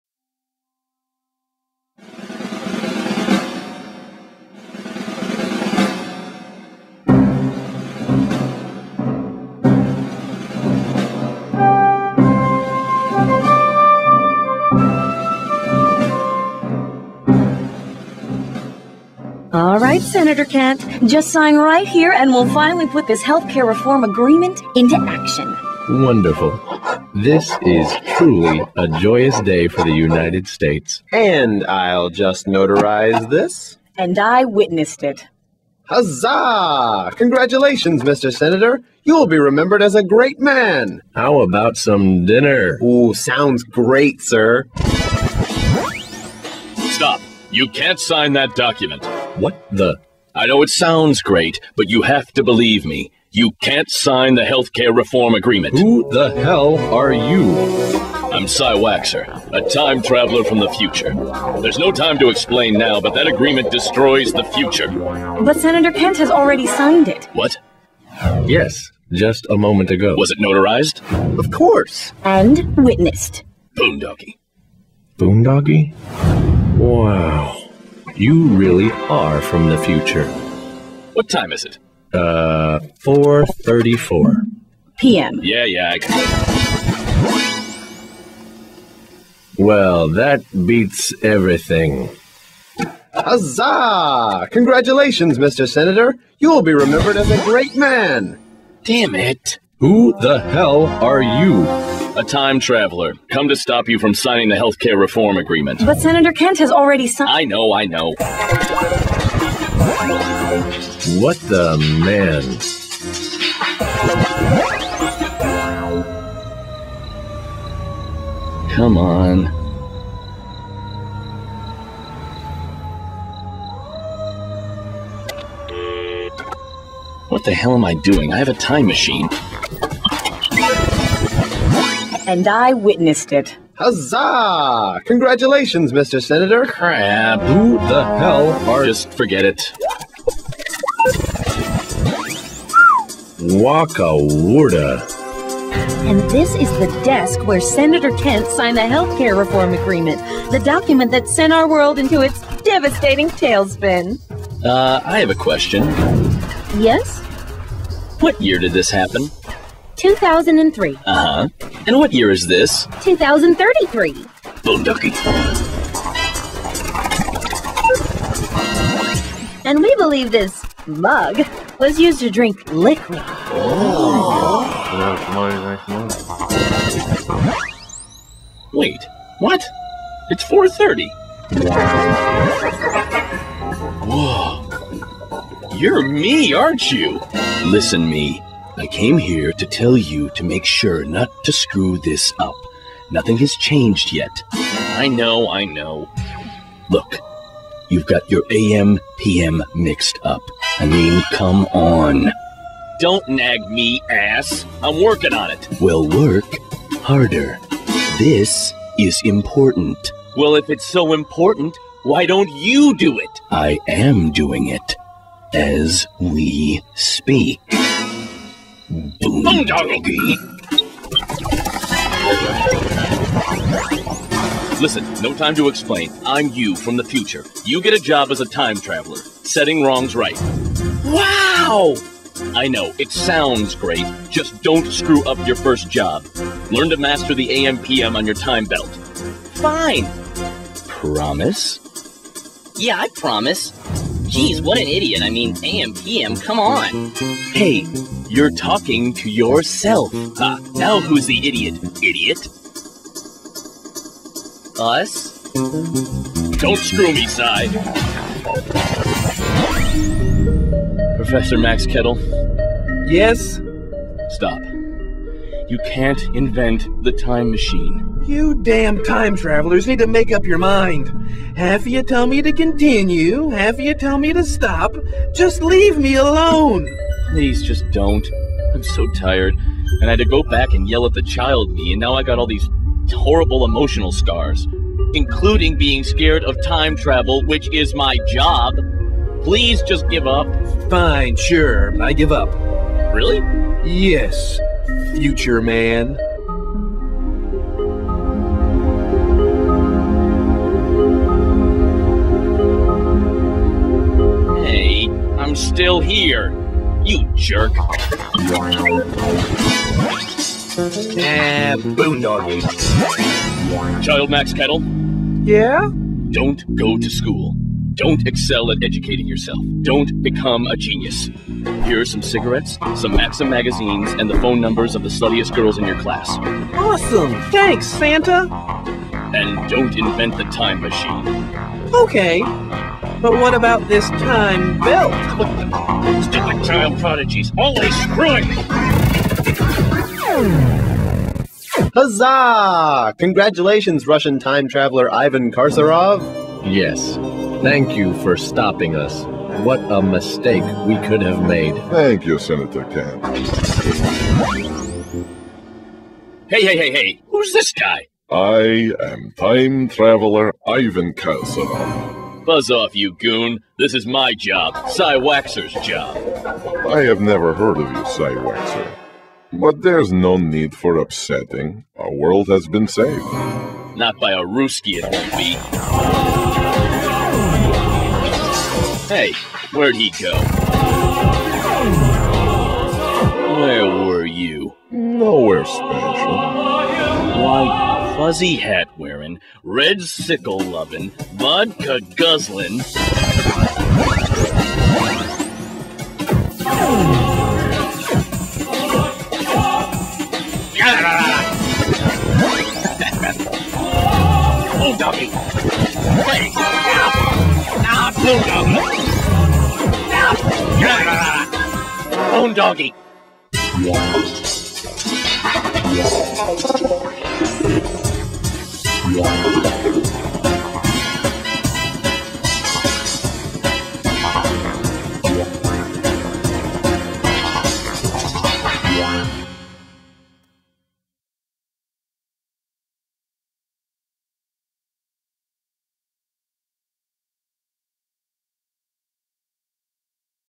Time Traveler
A time traveler sucks at his job.